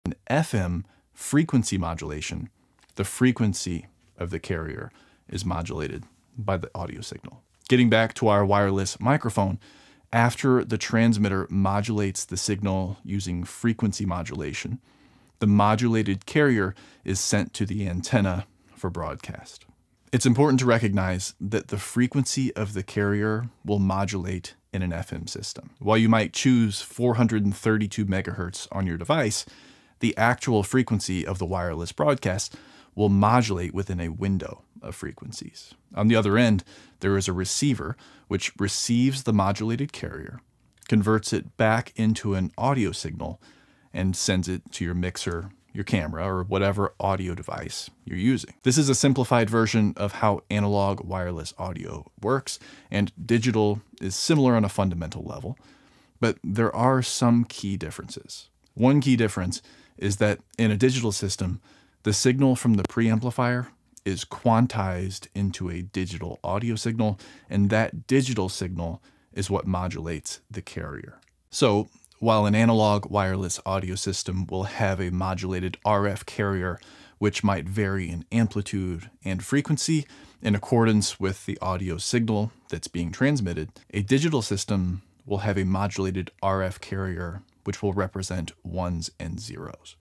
How FM Modulation Works in Wireless Mics
Explore how wireless microphones use FM to transmit audio!